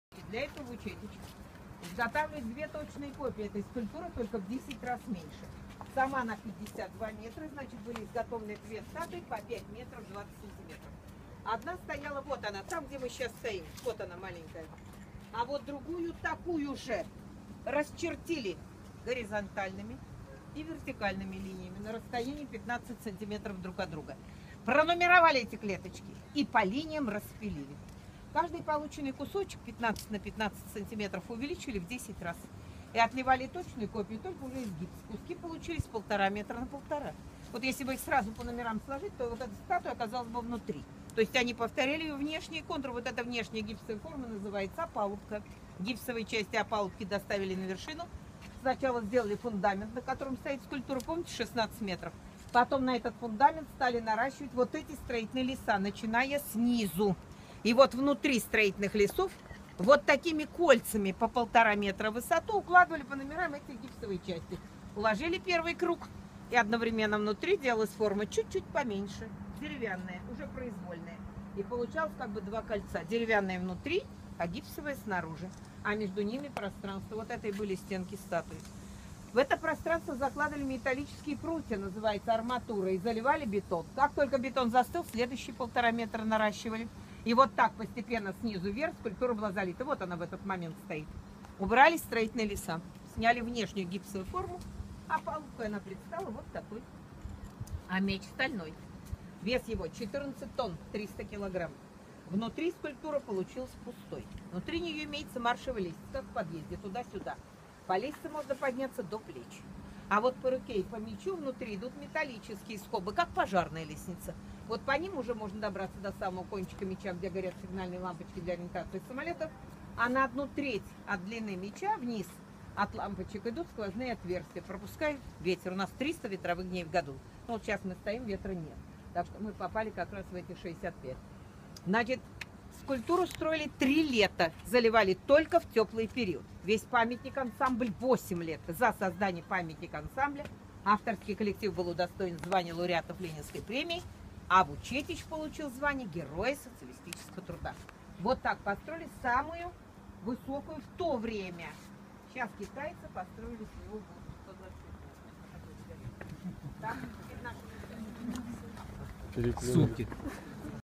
Ночная/вечерняя экскурсия по Мамаеву Кургану
Ночная/вечерняя экскурсия по историко-мемориальному комплексу "Героям Сталинградской битвы" на Мамаевом Кургане. Продолжительность экскурсии 1.5-2 часа.